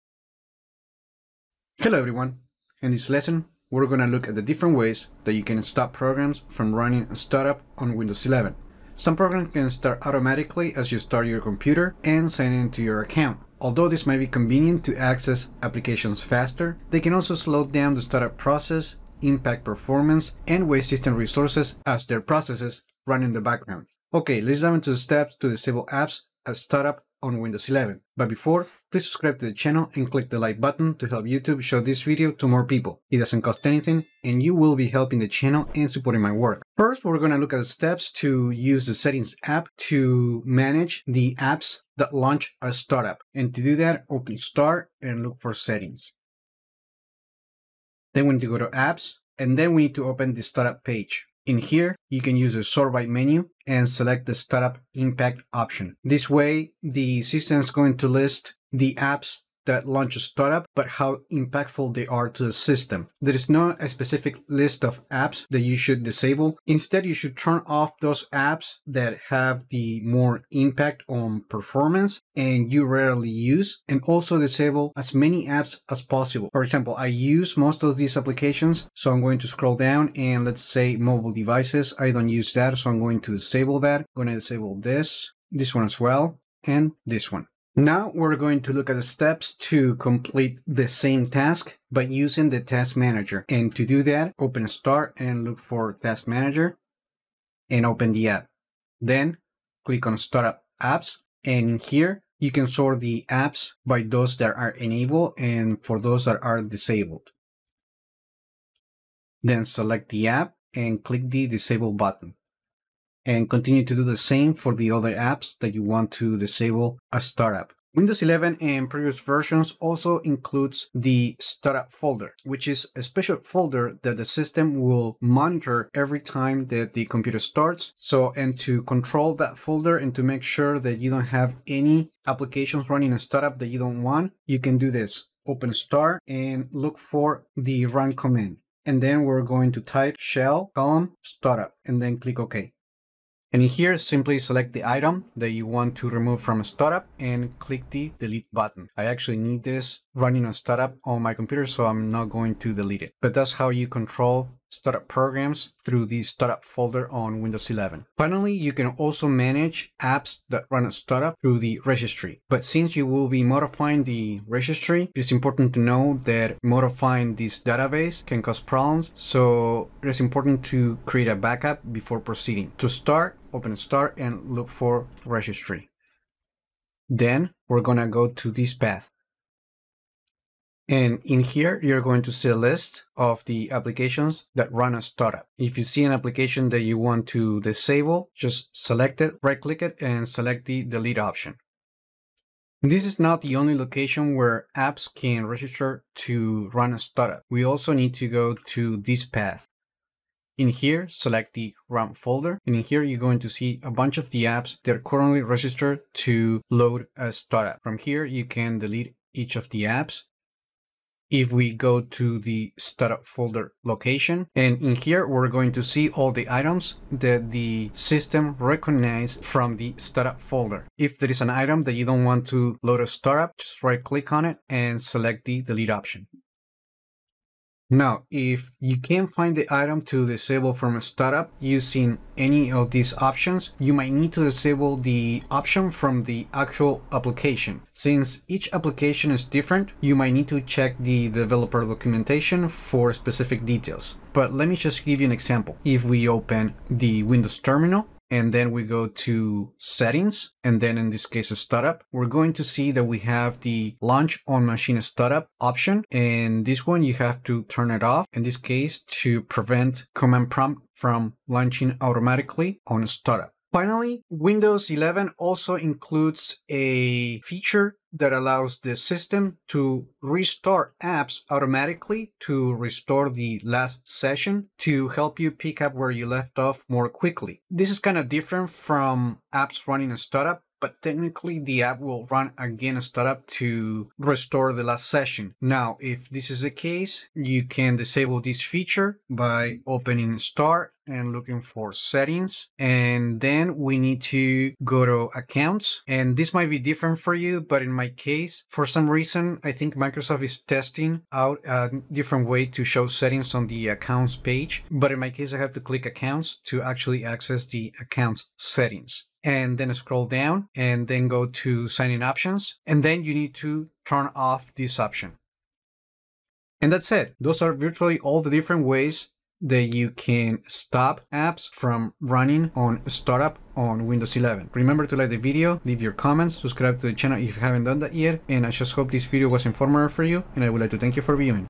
Windows 11: Stop programs from running at startup (6 Ways)
Windows 11 tutorial to disable apps at startup. Check Guide ✅
This video shows the steps to stop programs from running at startup from the Settings app, Task Manager, Startup folder, Registry, within the app settings, and disabling restartable apps on Windows 11.

00:00 Intro: Disable startup app
00:23 Disable startup apps via Settings App
01:28 Stop startup apps via Task Manager
01:58 Access and remove apps from Startup folder
02:38 Disable startup apps via Registry
03:48 Configure individual app startup settings
04:23 Disable restartable apps on Windows 11
05:16 Closing: Final tips for managing startup apps